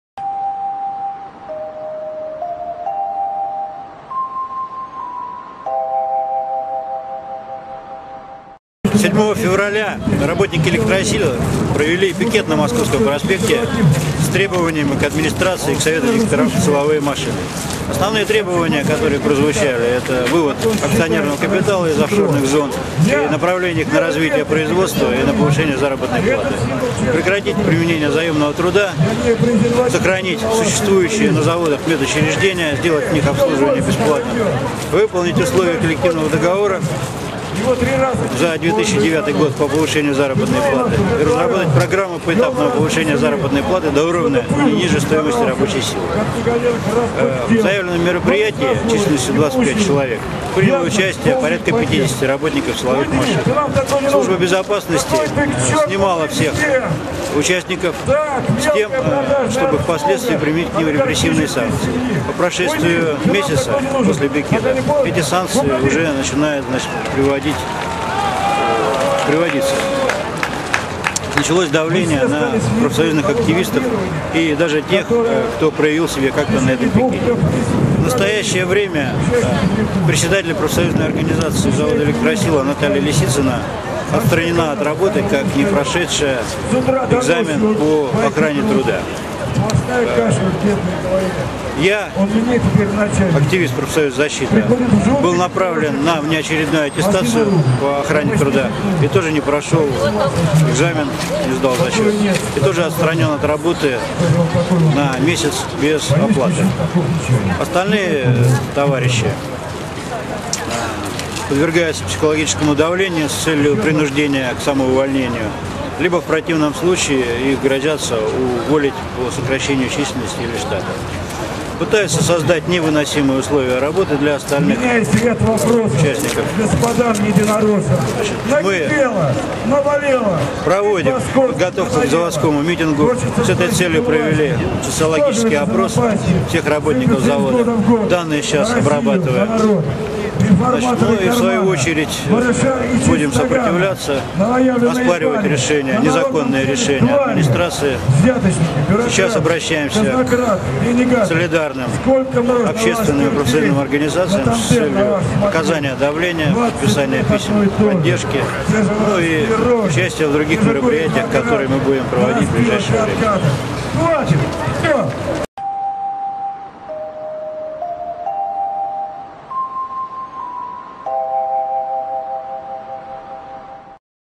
С.Маленцов о ситуации на заводе "Электросила" (17.03.2012)
Степан Маленцов - активист профсоюза "Защита" завода "Электросила"